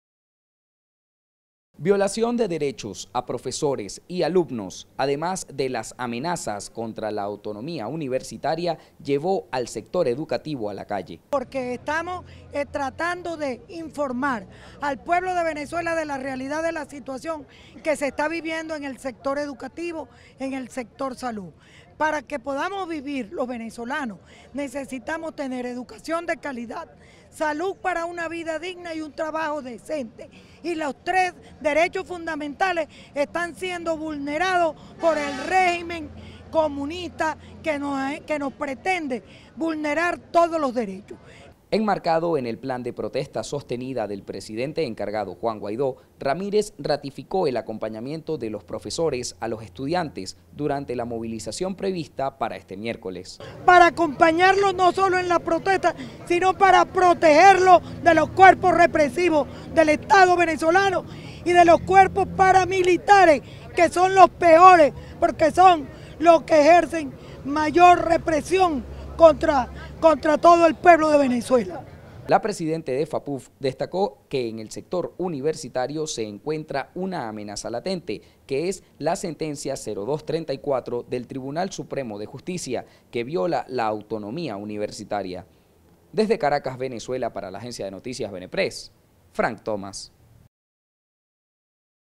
Profesores continúan en la calle
Violacion de derechos a profesores y alumnos además de las amenazas contra la autonomía universitaria llevó al sector educativo a la calle

Enmarcado en el plan de protesta sostenida del presidente encargado, Juan Guaidó, Ramírez ratificó el acompañamiento de los profesores a los estudiantes durante la movilización prevista para este miércoles.

La presidente de Fapuv destacó que en el sector universitario se encuentra una amenaza latente que es la sentencia 0234 del Tribunal Supremo de Justicia que viola la autonomía universitaria.